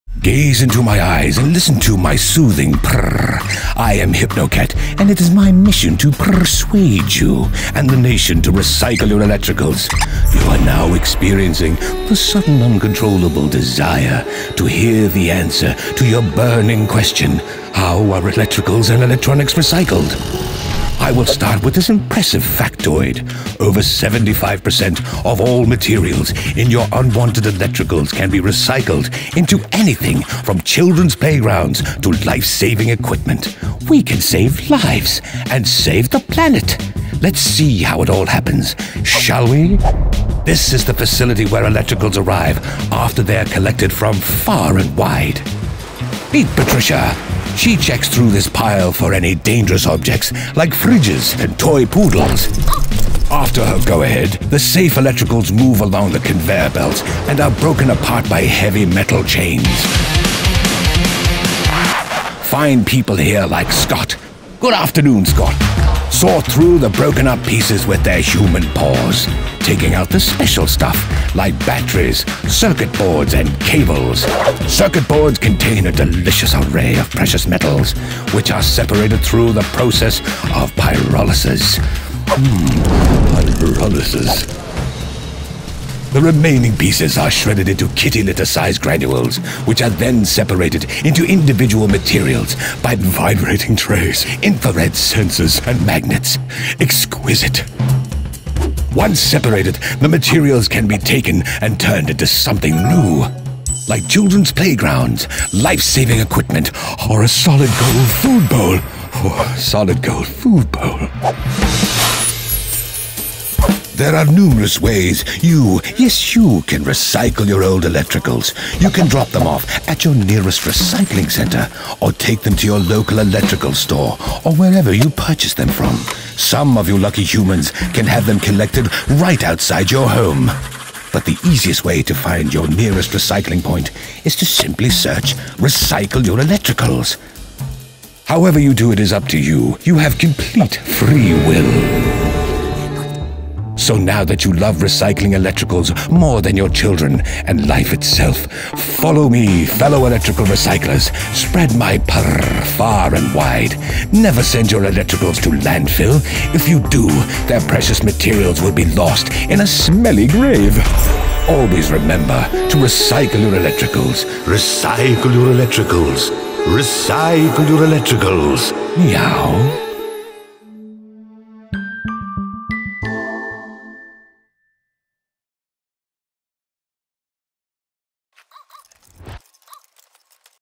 How are electronics recycled? Ask HypnoCat!
HypnoCat is a mesmerising pink fluffy cat with hypnotic superpowers and one mission - to hypnotise the UK into recycling their electricals!

If you've ever wondered what happens when you recycle your old electrical items - wonder no more! HypnoCat is here to explain the whole process from top to tail.